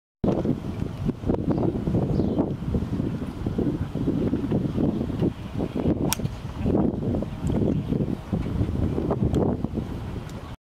Rory McIlroy Fairway Wood Swing at the Memorial Tournament 2014 (May 27, 2014)
During Tuesday practice round (1st hole) at the Muirfield Village Golf Club (Dublin, OH).